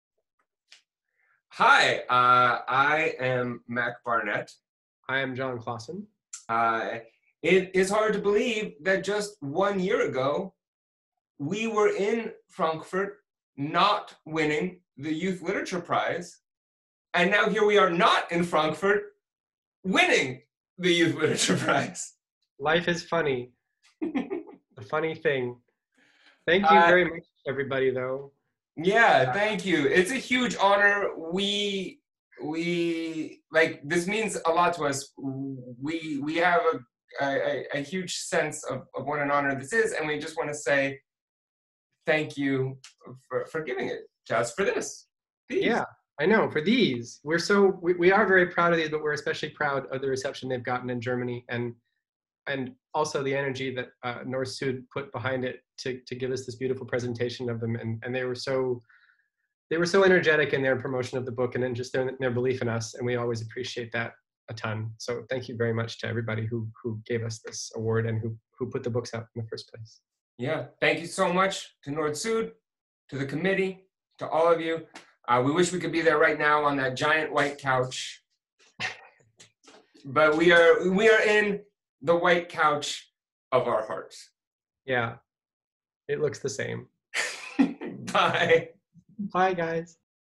DJLP - Die Gewinner melden sich zu Wort
Jon Klassen und Mac Barnett bedanken sich! Mit "Dreieck Quadrat Kreis" haben die beiden den Deutschen Jugendliteraturpreis gewonnen.
Herzlichen Glückwunsch!